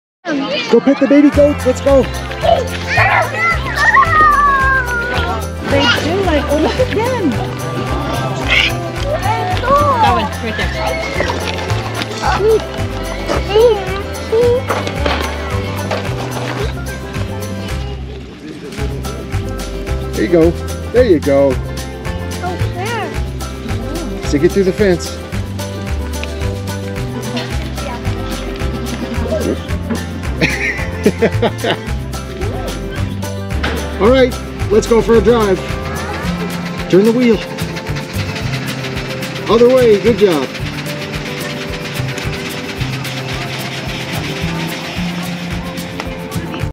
A trip to the Calgary Farmyard!!!
We visit the Calgary Farmyard & Petting Zoo for some family fun!